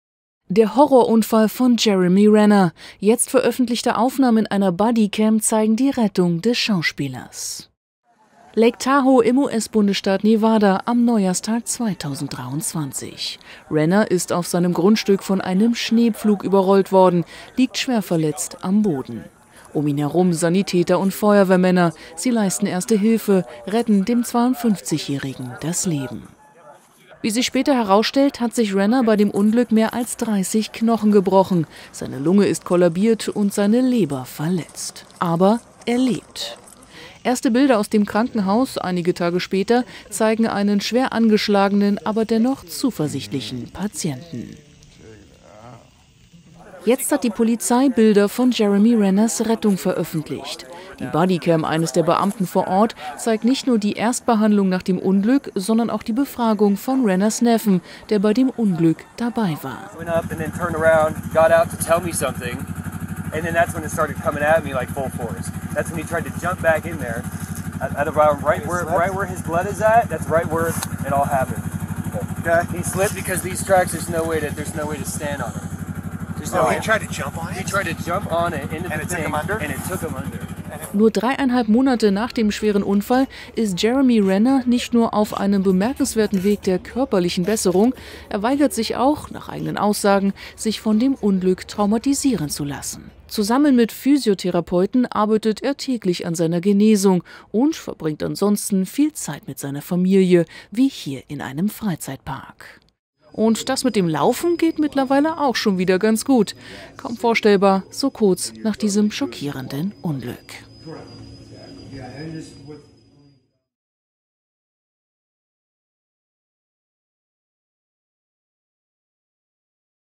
Jeremy Renners Horror-Unfall – Bodycam-Aufnahmen zeigen dramatische Rettung
Der Horror-Unfall von Jeremy Renner – jetzt veröffentlichte Aufnahmen einer Bodycam zeigen die Rettung des Schauspielers.

Der brutale Unfall ereignete sich am Neujahrstag dieses Jahres, als Renner von einem sieben Tonnen schweren Schneepflug überfahren wurde. Der Hollywood-Star hatte versucht, die plötzlich los rollende Maschine zu stoppen.

„Ich würde es wieder tun“, sagt Renner heute, „Das Ding rollte direkt auf meinen Neffen zu.“ Wie durch ein Wunder überlebte Jeremy Renner den lebensgefährlichen Unfall, erlitt allerdings massive Verletzungen – brach sich über 30 Knochen, hatte Quetschungen innerer Organe, zudem kollabierte seine Lunge.

Knapp drei Monate nach seinem schweren Unfall zeigte sich der Schauspieler im April erstmals wieder auf dem roten Teppich. Ein Wahnsinns-Comeback, das zeitweise keiner mehr für möglich hielt.